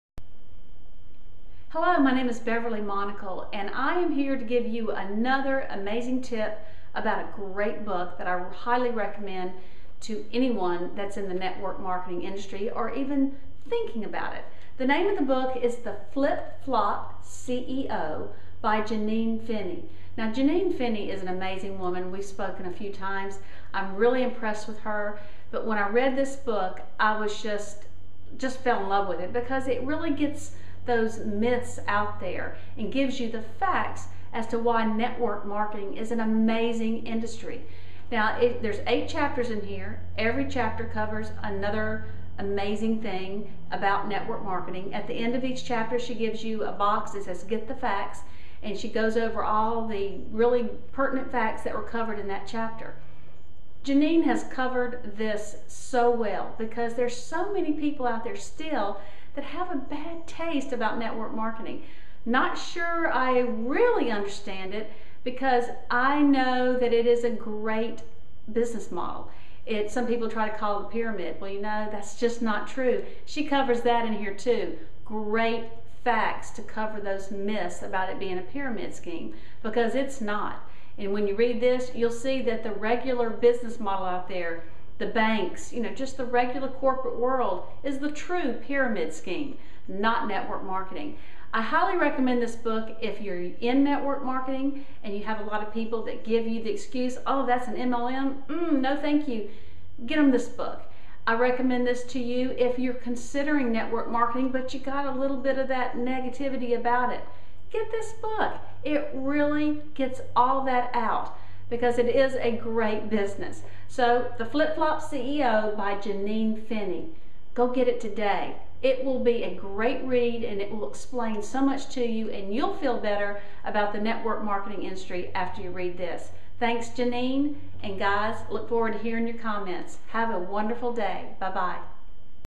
Should You Buy The Flip Flop CEO?
Beverly Monical is letting you know if you should buy The Flip Flop CEO book by Janine Finney. Watch and see if she recommends it.